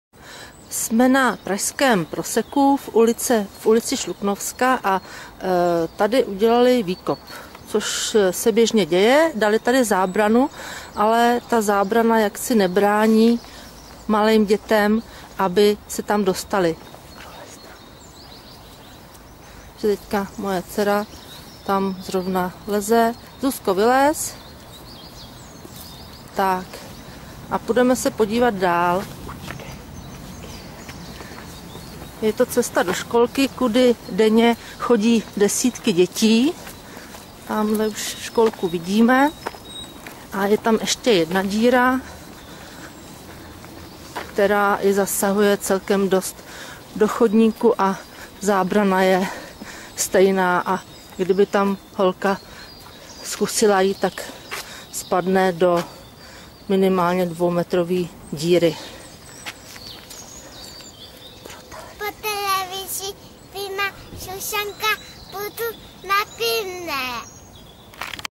Zábrana, která nebrání
Natočil jsem výkopy, které jsou ohraničeny zábranou aby do výkopů nikdo nemohl spadnout, ale mezery v zábranách jsou tak velké, že bez problému proleze malé dítě, možná i hubenější dopsplý. Vzhledem k tomu, že výkopy jsou hned před mateřskou školkou, je to velmi nebezpečné. - Autor: Vojtěch Duchoň